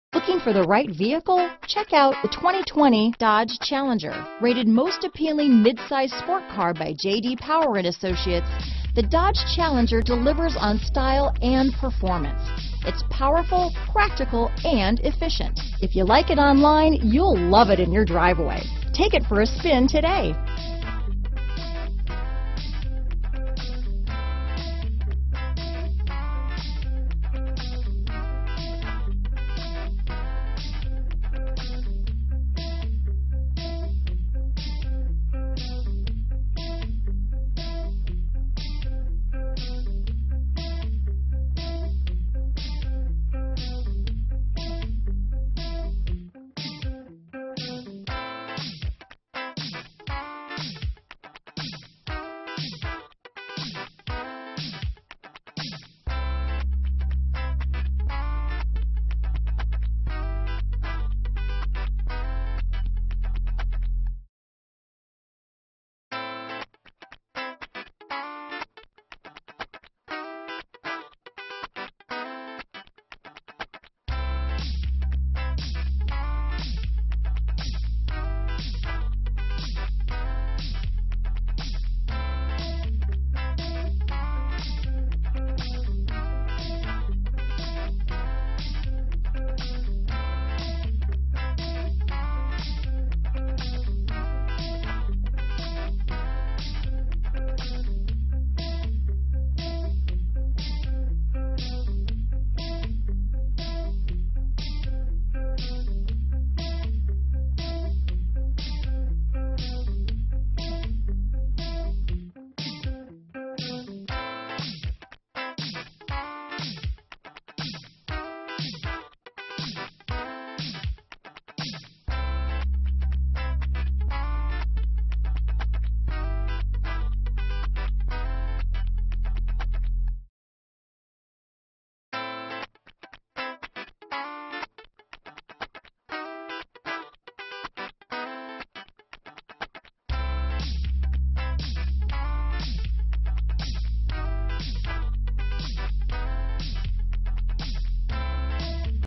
2020 DODGE CHALLENGER Redding, Eureka, Red Bluff, Northern California, Sacramento, CA 20D127
WHITE  KNUCKLE New 2020 DODGE CHALLENGER available in Redding, California at Crown Motors Redding. Servicing the Eureka, Red Bluff, Northern California, Sacramento, CA area.
2020 DODGE CHALLENGER  - Stock#: 20D127 - VIN#: 2C3CDZFJ6LH184502